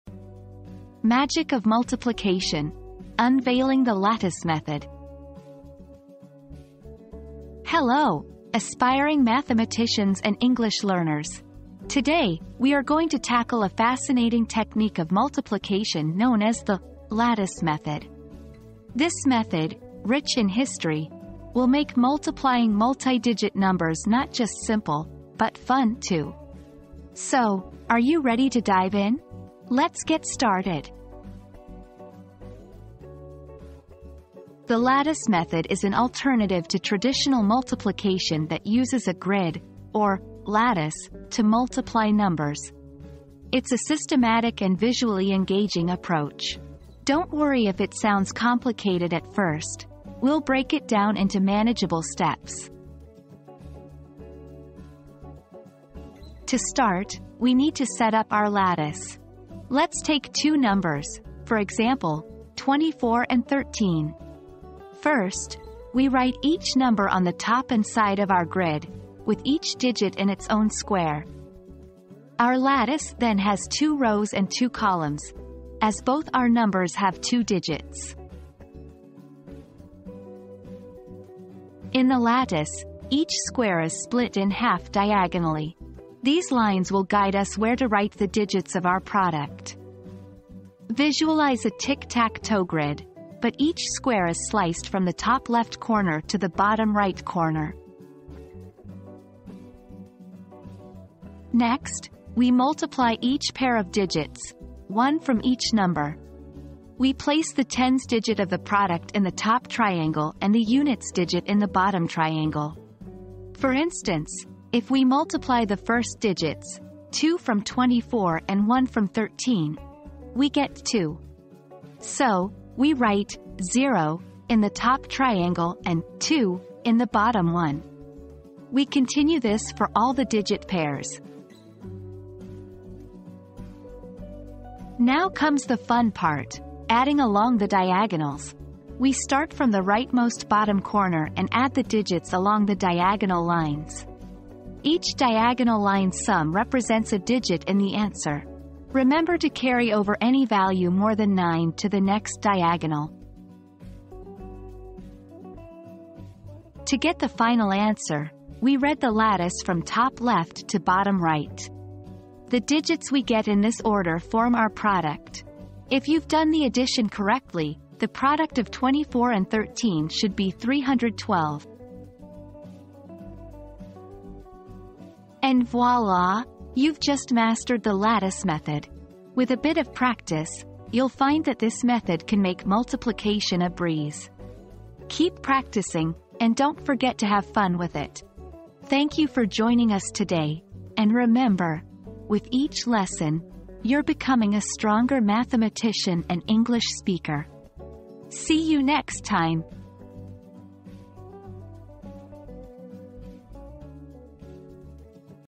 Magic of Multiplication: Unveiling the Lattice Method!
The Magic of Multiplication: Discover the Lattice Method! • Learn the fascinating and visually engaging Lattice Method of multiplication. Follow along as we break down the steps and show you how to multiply multi-digit numbers with ease. Get ready to unleash the magic of multiplication!

00:00 • Introduction - Magic of Multiplication: Unveiling the Lattice Method!
00:34 • Understanding the Lattice Method
00:55 • Setting up the Lattice
01:21 • Drawing Diagonal Lines
01:40 • Multiplying and Filling the Lattice
02:14 • Adding Along the Diagonals
02:36 • Reading the Answer